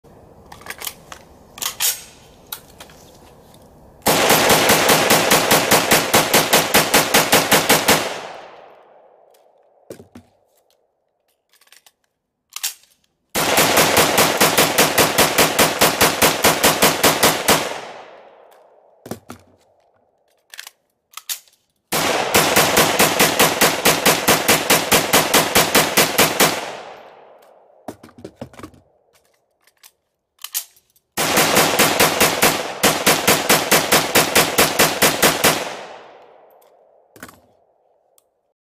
SKS Rapid fire super rapid release mag system
SKS using the Super Rapid Release from Evan Jenkins at Texas Firearms, 4 20 round mags back to back